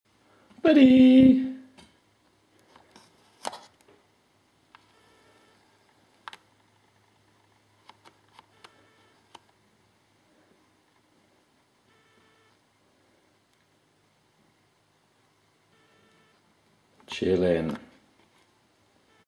Chihuahua "Buddy" Chilling
My new Chihuahua was very nervous and shaky when I first got him , now after a couple of days he is starting to come out of his shell and seems a lot happier :)